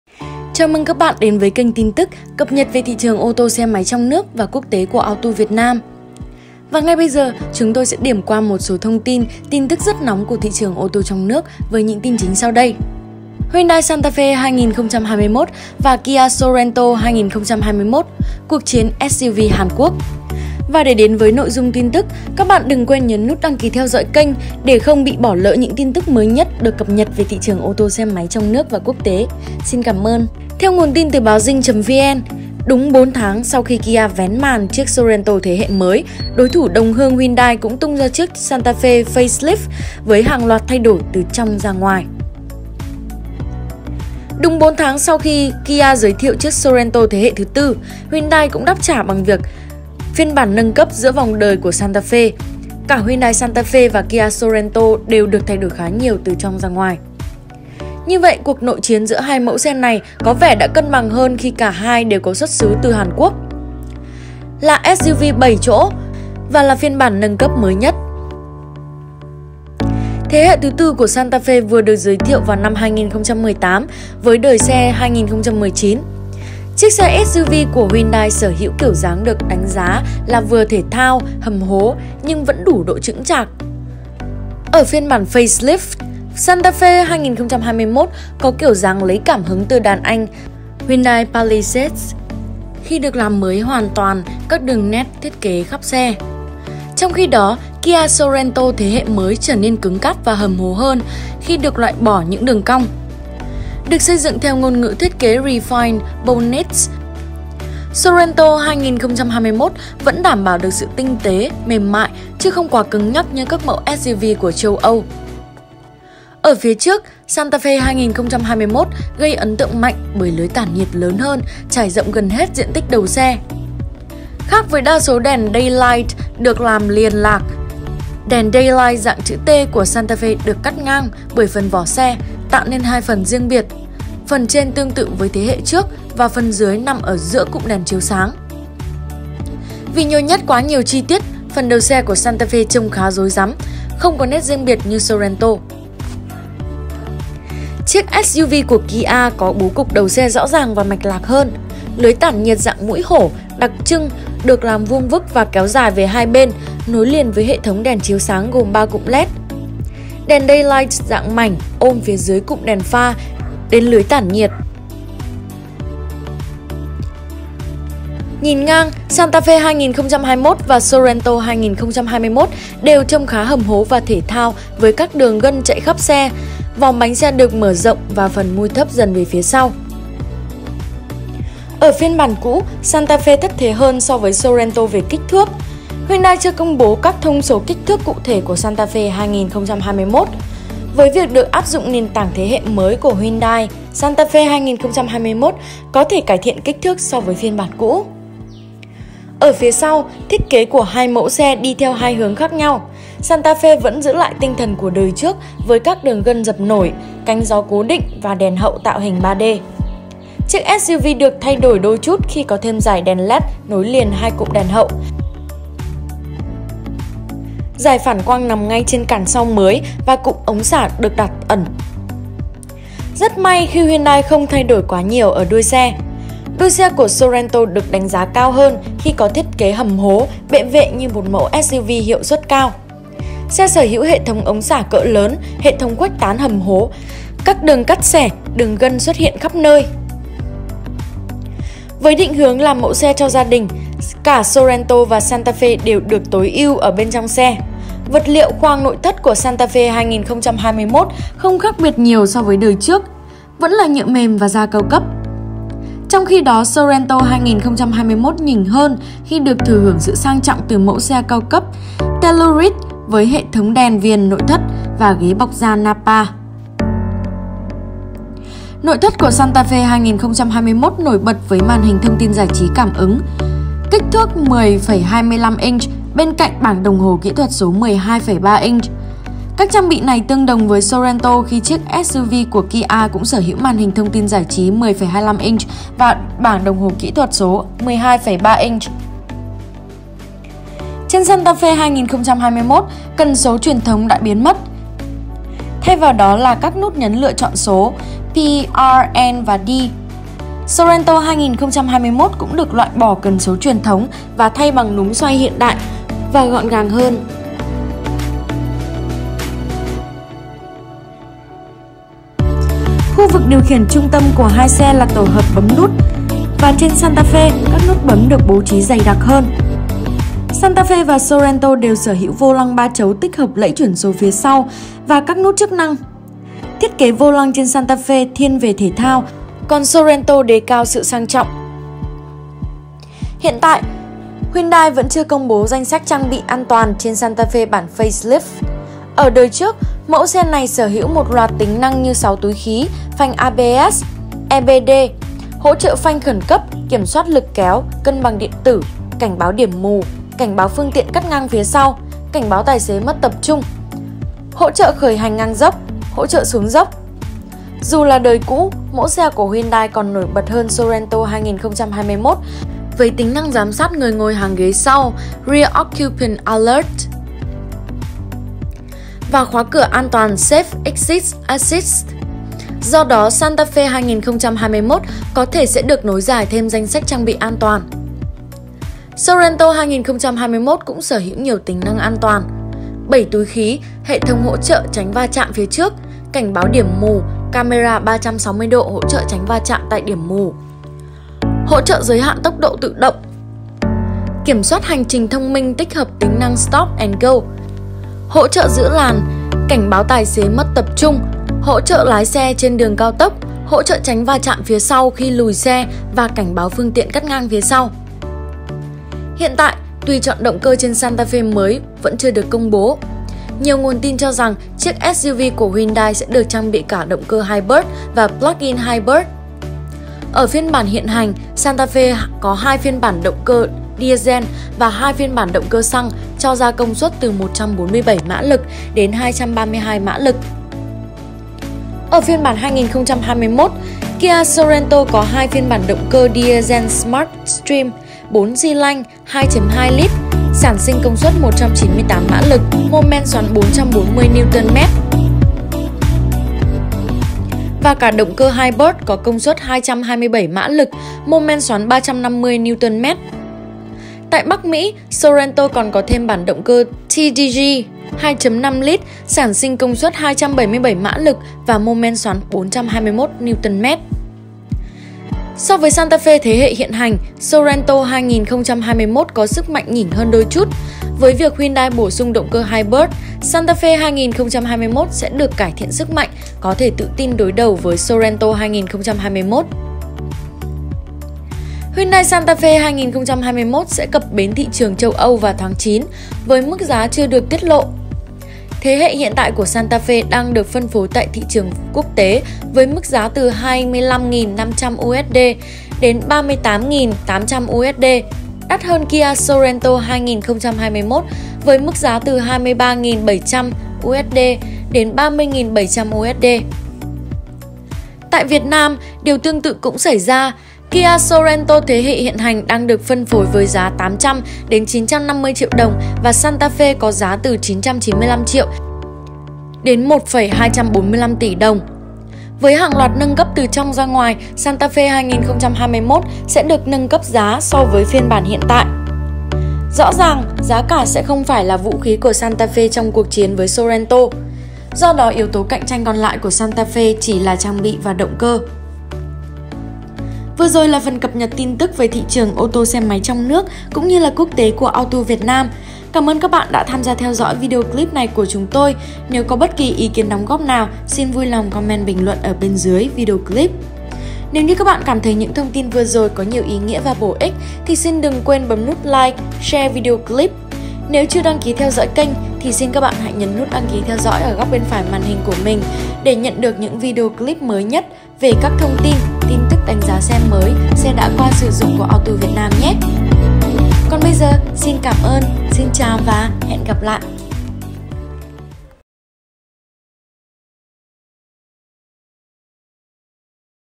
✅So sánh Hyundai Santafe 2021 và Kia Sorento 2021.
-Nguồn tin: Zing.vn
-Hyundai Santafe  là một trong những mẫu xe rất thành công ở tại thị trường Việt Nam. Kia Sorento lại là một trong những mẫu xe không mấy thành công ở tại Việt Nam, tuy nhiên với những model 2021 , dường như hai mẫu xe này đang có vẻ cân tài cân sức!